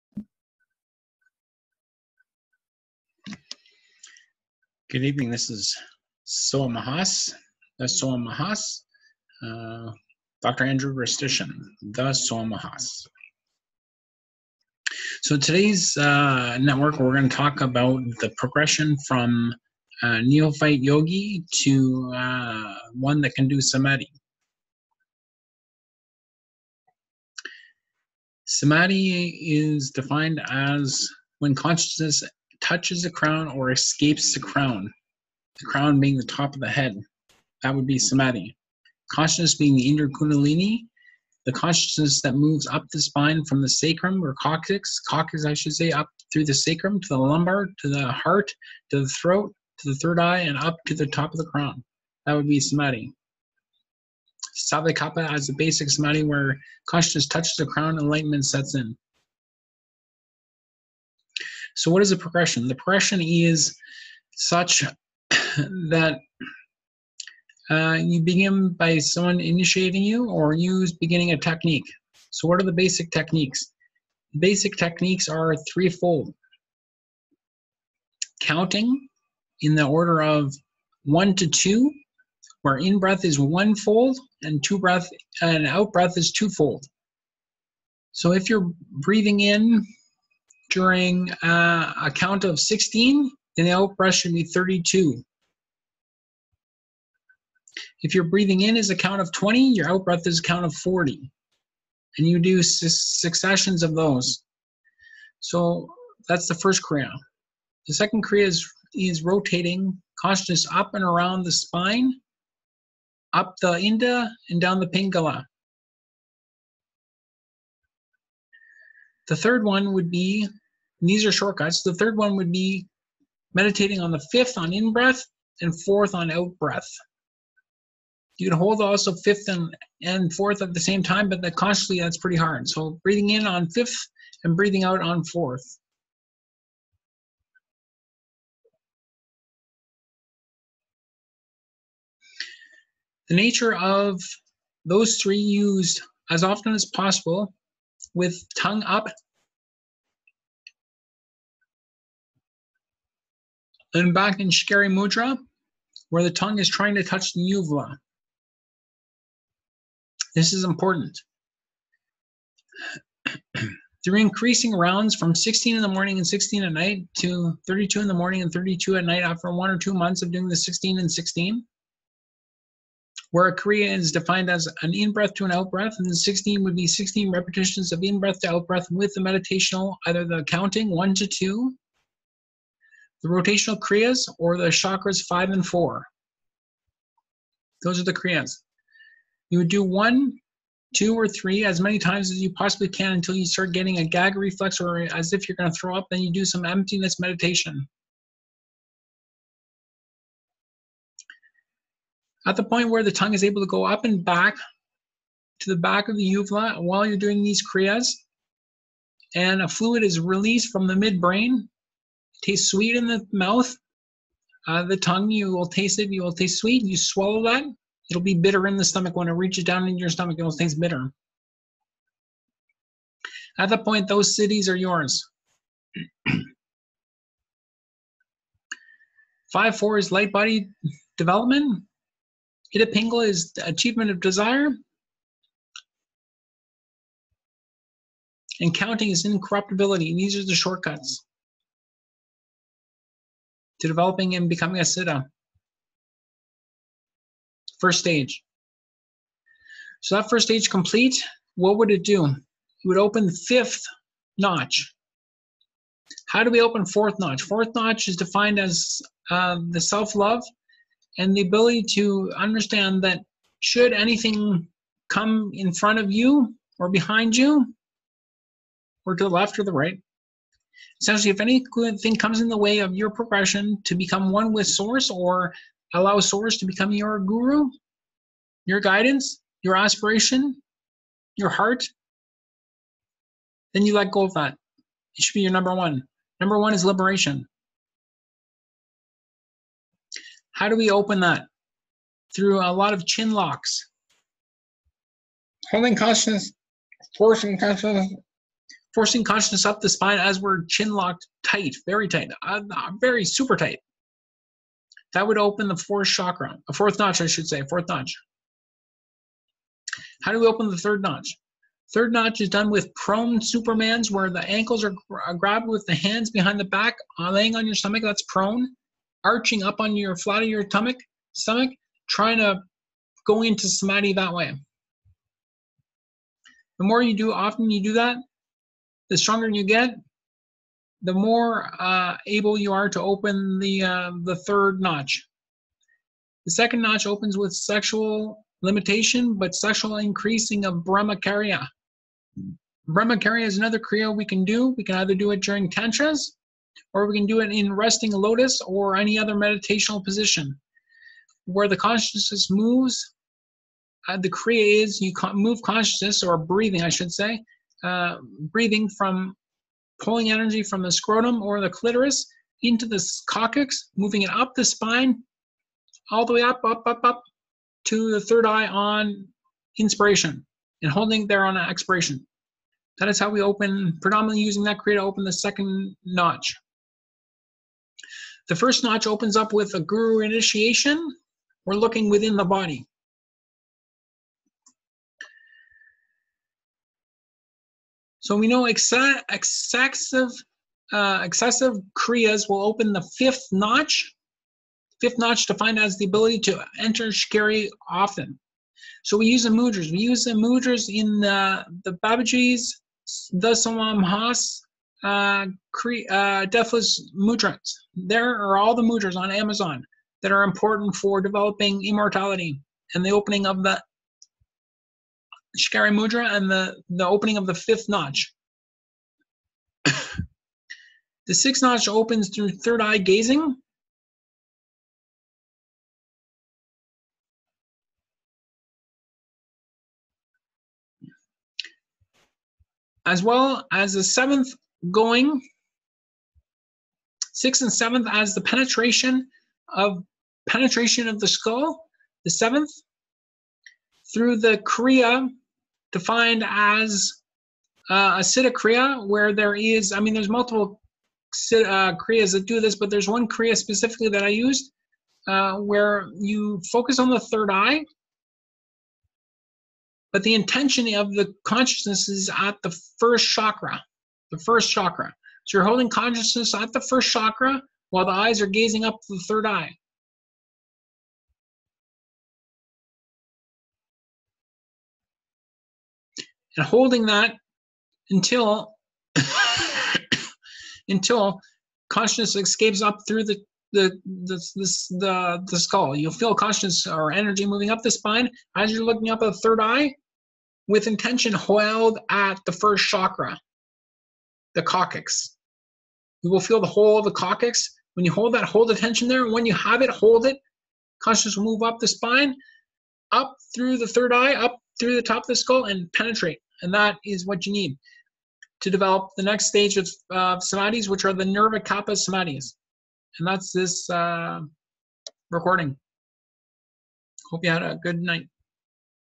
Short Cuts To Prepare For Development of Samadhi
Use this video to help prepare for the development of Samadhi. Explains the nature of which yogas do what to open the "notches" those explained within the Book of Revelation (see Book of Revelation Code by Greszczyszyn for more if desired) by Jesus Christ!